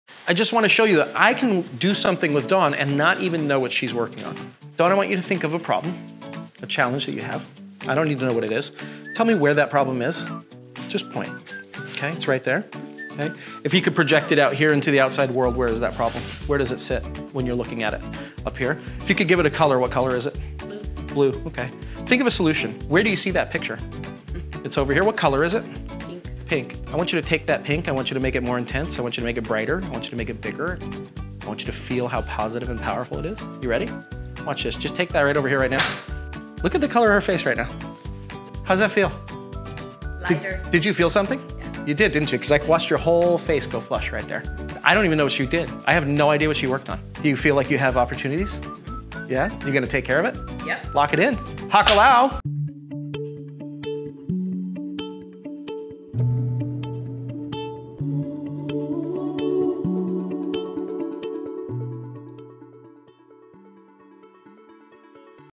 See How NLP Can Change Your Perception of Problems | Transform Destiny | Michael Stevenson
Transforming Problems into Powerful Solutions! Imagine being able to visualize your challenges as colors and actually intensify and brighten the solutions. Learn how to unlock your hidden potential with our NLP techniques and take control of your life!